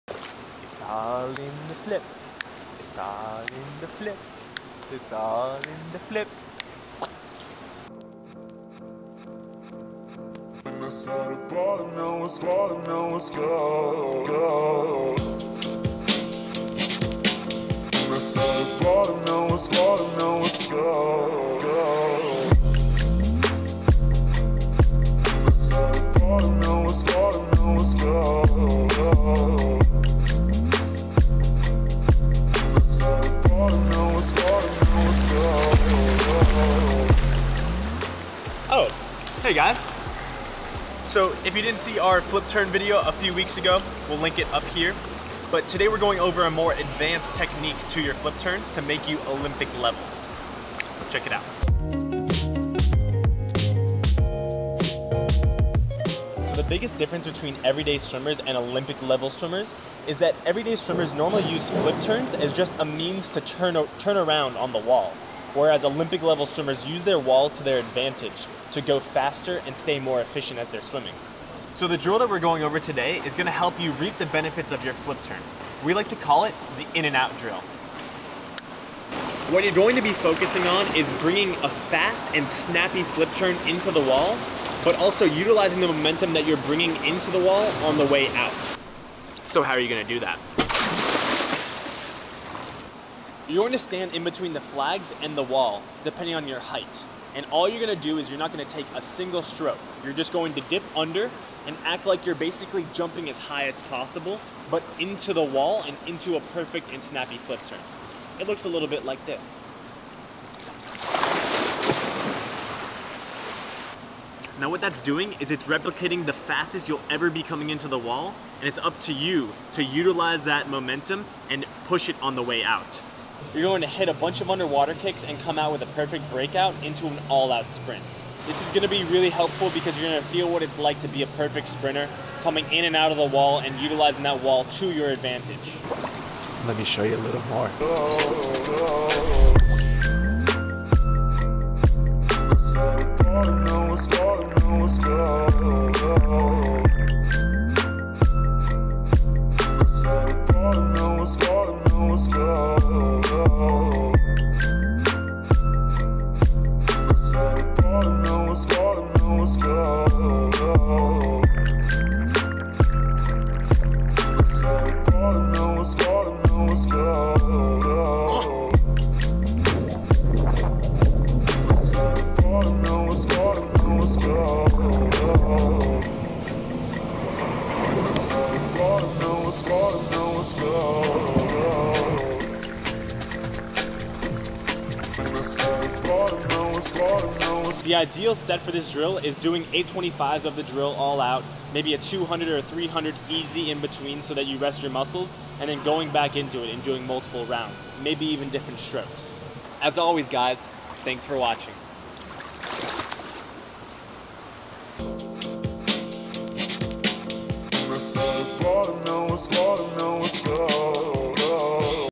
THE SECRET TO FLIP TURNS
This week we wanted to focus on a problem that we see on pool decks across the globe. Most swimmers use their walls as a means to turn around, but not to the full extent of using them to your advantage. The ‘In-N-Out’ drill helps you do exactly that. A quick and easy drill that replicates the power of top speed and teaches you to utilize momentum on your way into the wall, and absorb it out.

As usual, Let us know what you think of this weeks drill! Recommendations for our next video topic?